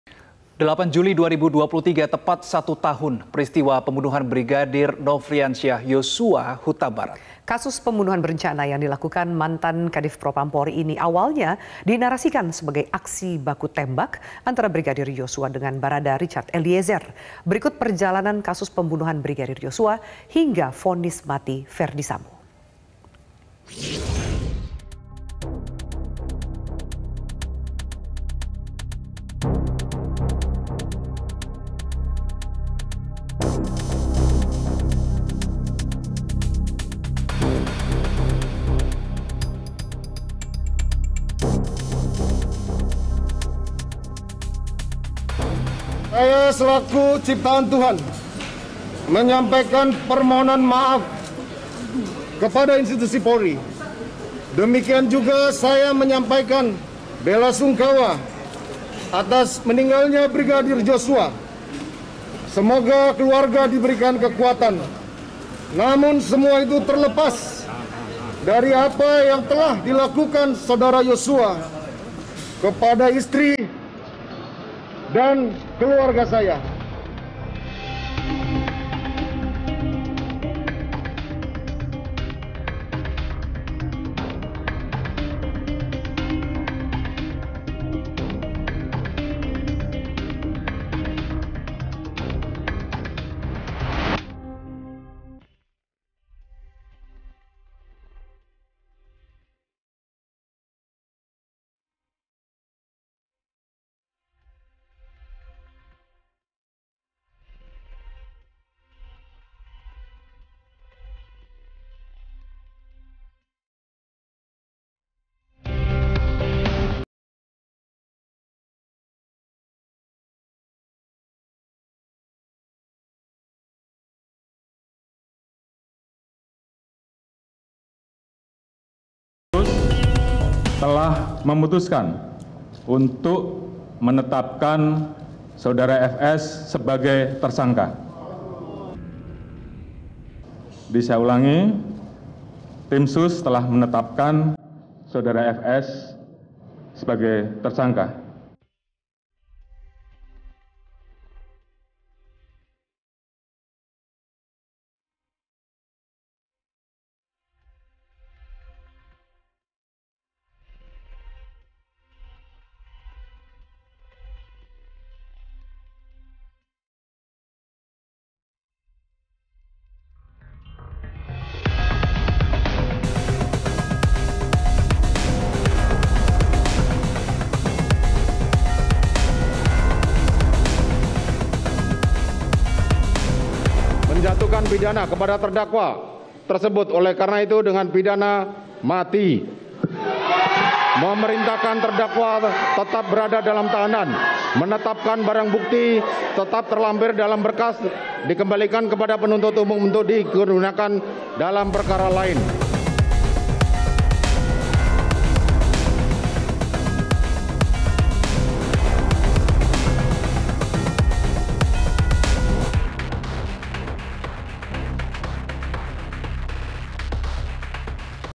Setahun Brigadir J Dieksekusi di Rumah Ferdy Sambo
MetroTV, setahun peristiwa eksekusi Brigadir J menghasilkan rangkaian drama panjang antara Ferdy Sambo CS, dan Keluarga Brigadir J.